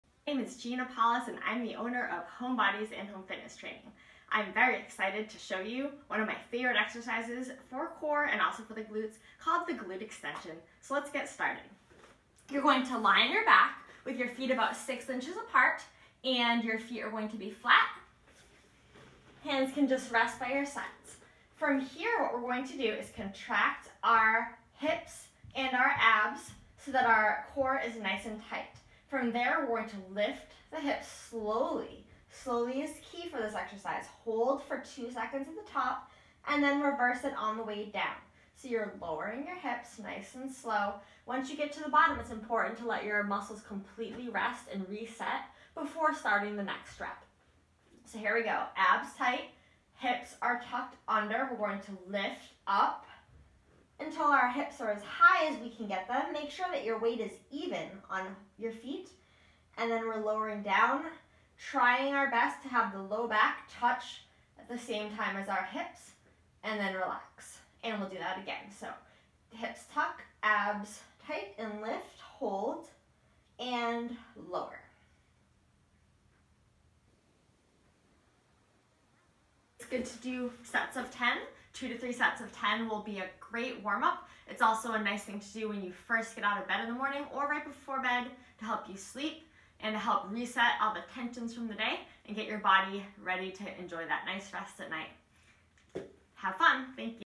Glute bridge
This exercise helps tone the glutes, core and hamstrings. For more videos like this check out my home exercise library